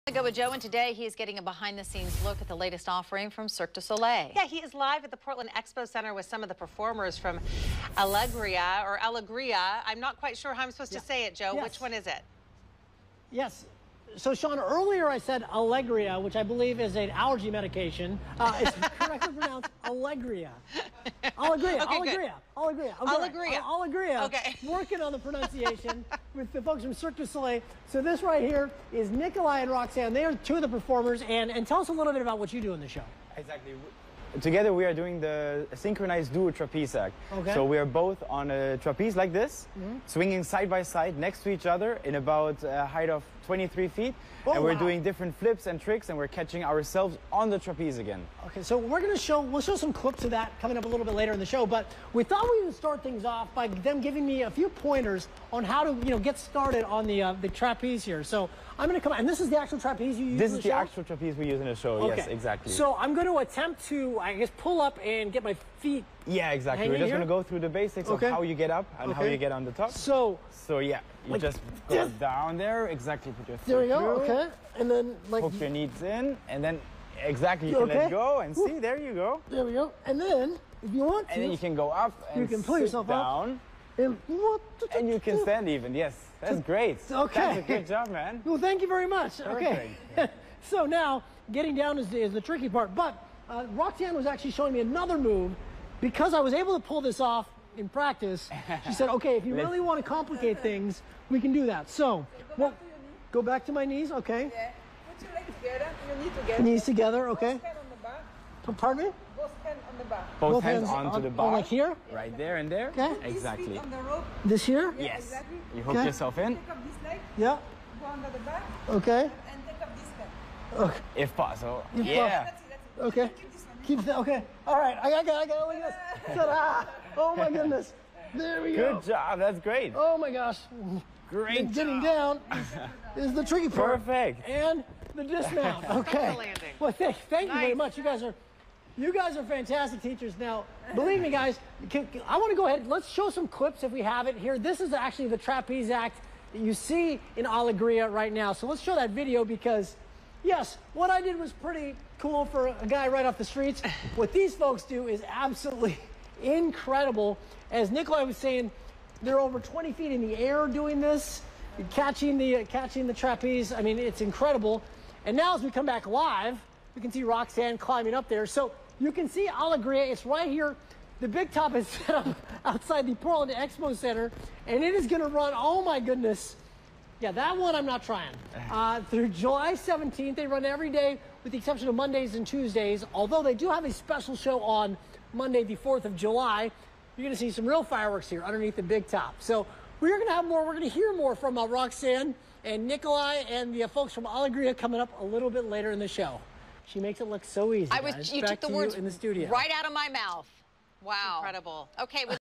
On the Go with Joe at Cirque du Soleil Alegría
Cirque du Soleil has returned to the Rose City with some high-flying action!